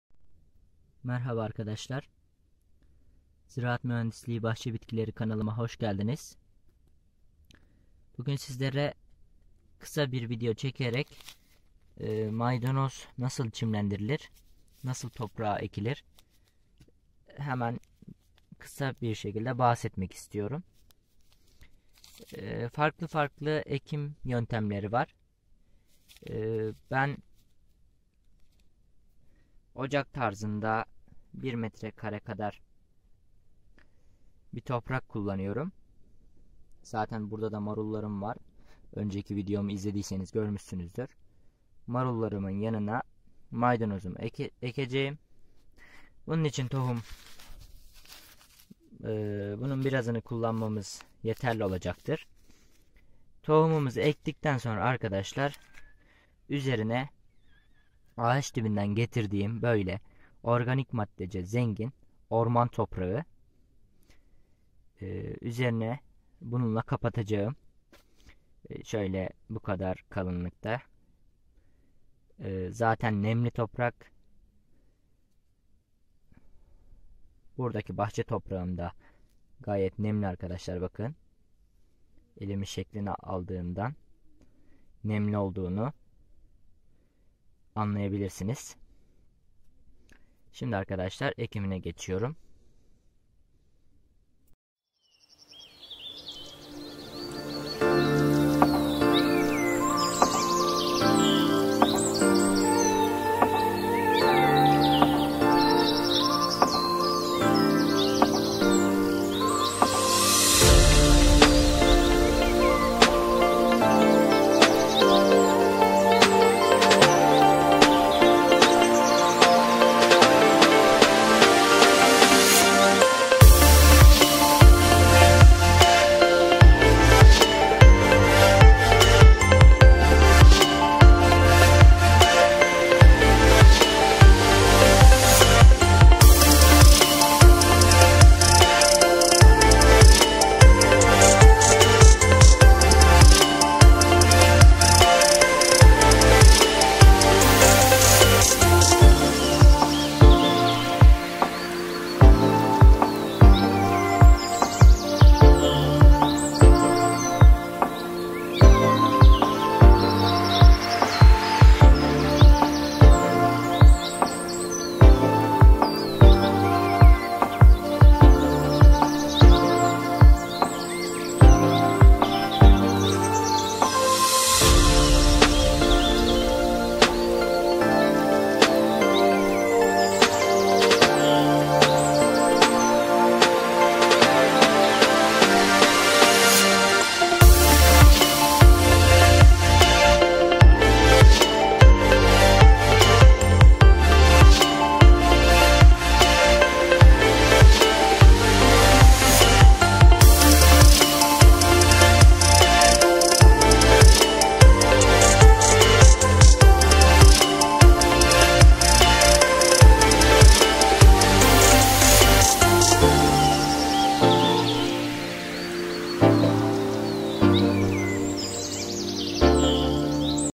👉 MAYDANOZ TOHUMU NASIL ÇİMLENDİRİLİR?
- Maydanoz tohumu roka, tere vb gibi tohumlardan biraz daha geç çimlenir...
- Toprağın nemine bağlı olarak sulama yapabilirsiniz.
- Yaz, kış yetişebilen bir bitki olduğu için bakımı oldukça kolaydır.
- Video hakkında aklınıza takılan bir soru olursa yorum kısmına bırakmanızı rica ediyorum.
- Teşekkür ederim. 😊